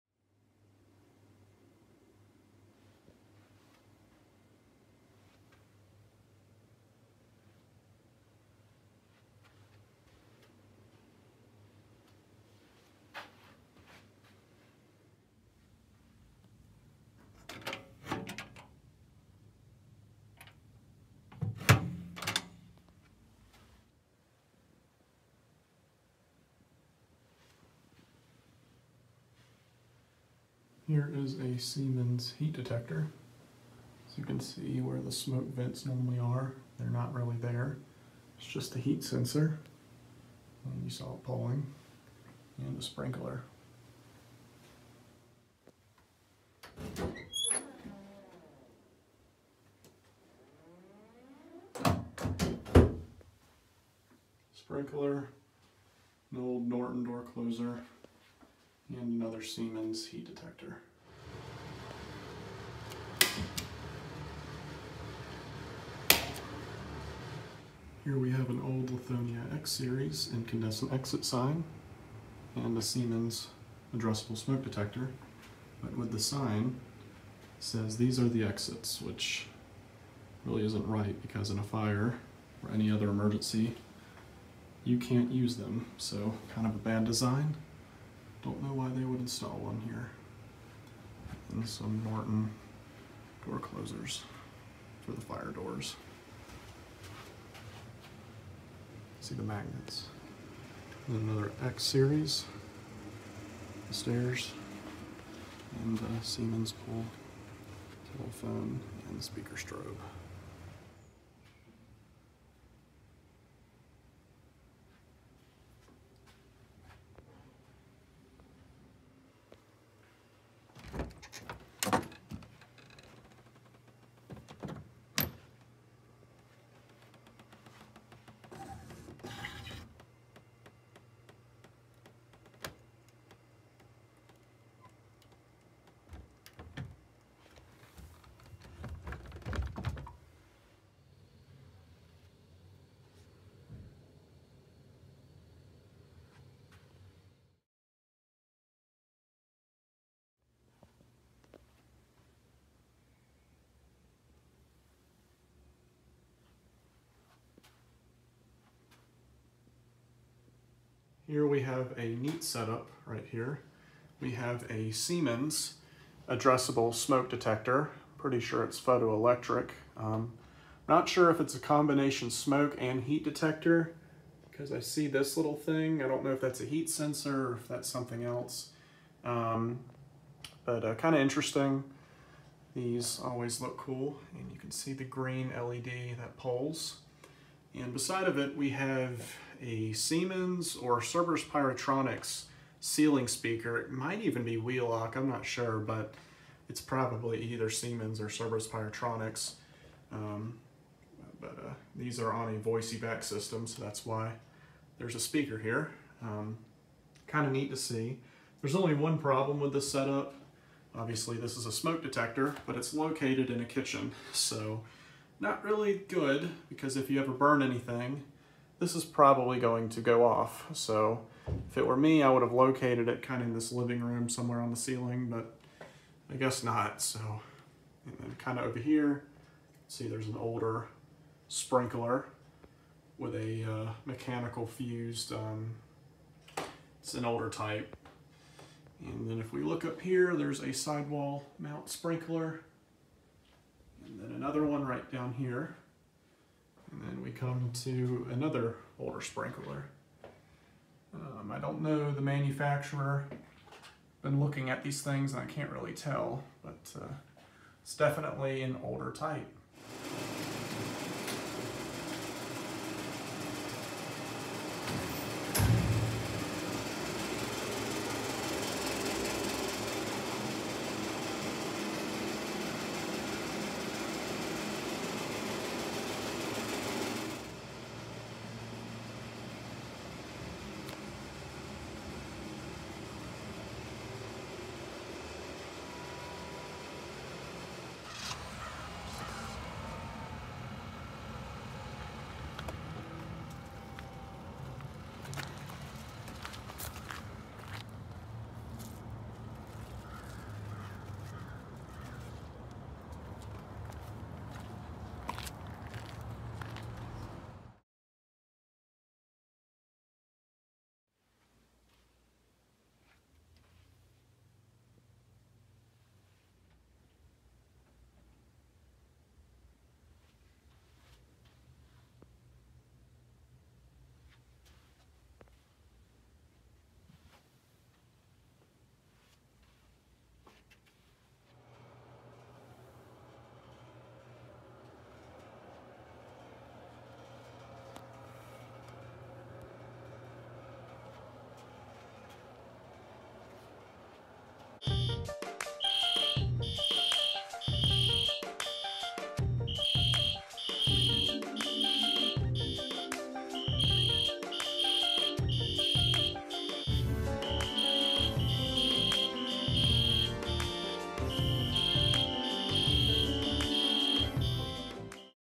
Out and About with ExitSign250 | Episode 7
In this Out and About video, I find some fails like exit signs directing to elevators and a smoke detector in a kitchen. I also find some vintage fire safety equipment and other neat stuff!

©️ExitSign250 - - Do not use any content featured without permission.